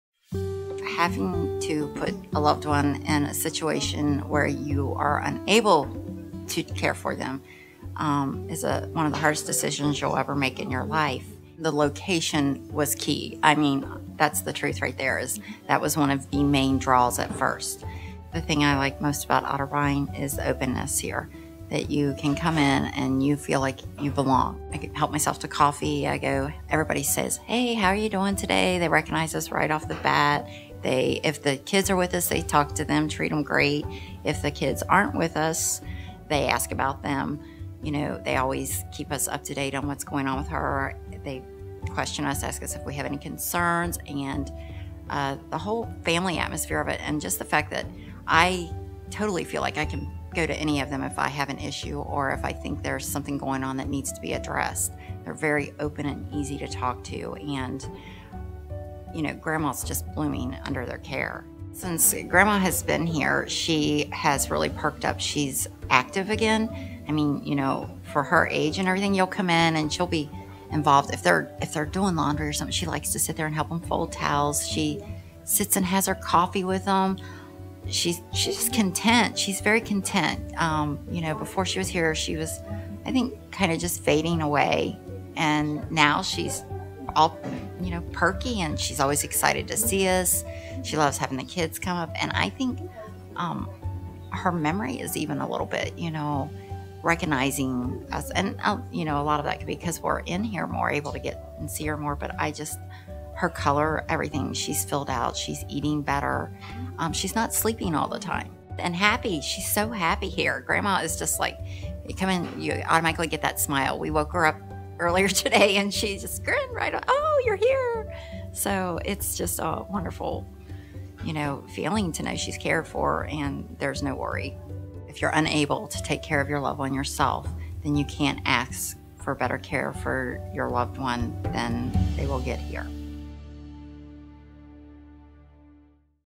Small House Big Difference - Otterbein Cares
Here's what one resident's family member has to say about the care that is given to her grandma at one one of the Otterbein Neighborhoods.